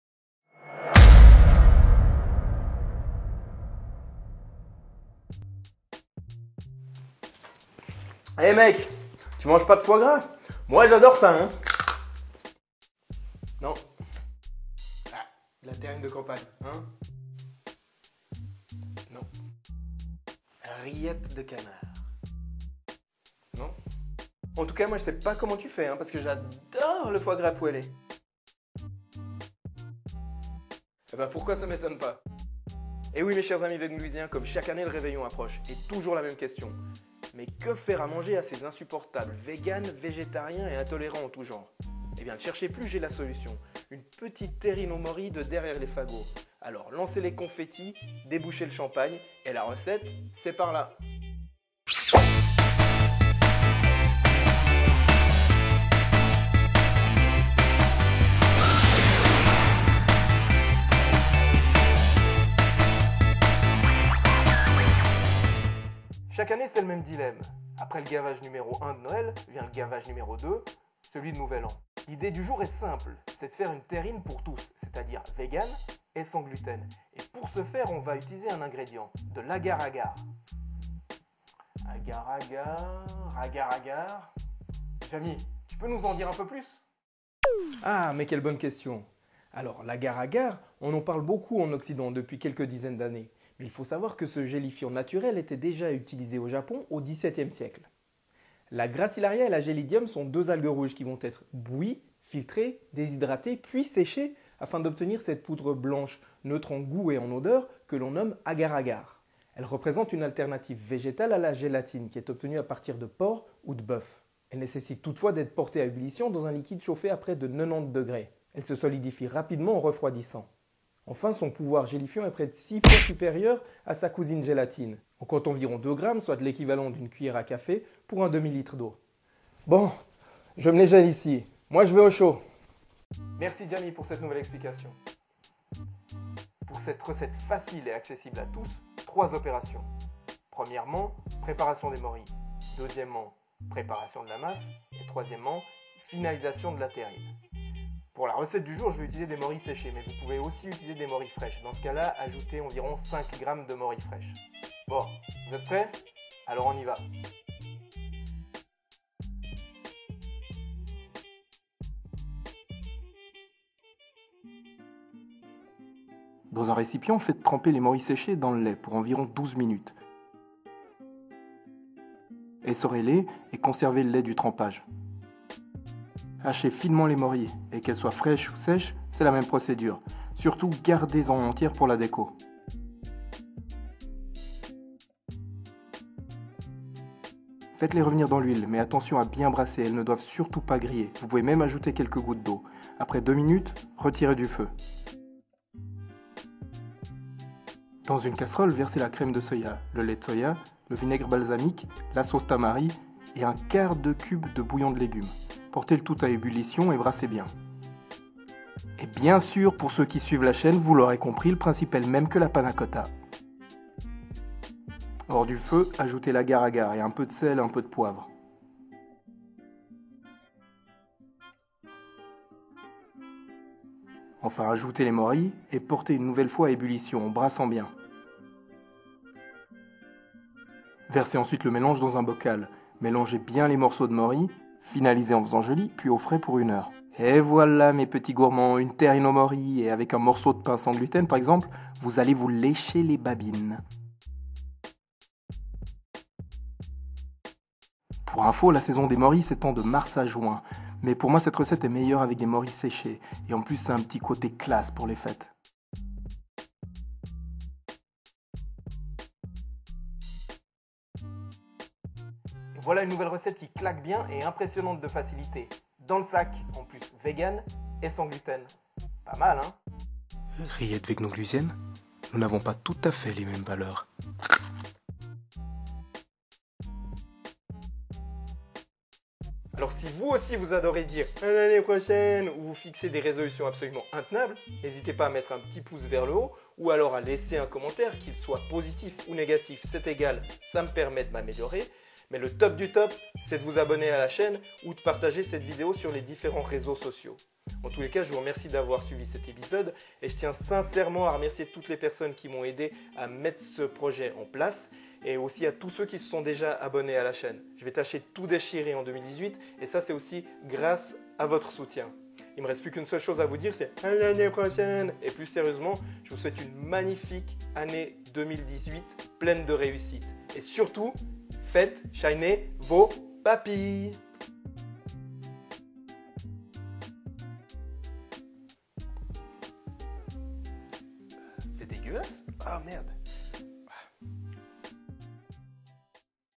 Terrine aux Morilles (E10)
Terrine aux Morilles (Gluten free et Vegan)

2-3 personnes
20 Min

Morilles séchées 15g
Lait de soja 2 dl

Crème de soja 1.2 dl

Vinaigre balsamique 1 CS
Sauce Tamari 1 CS
Bouillon de légumes en cube 1/4
Agar-agar ½ cc rase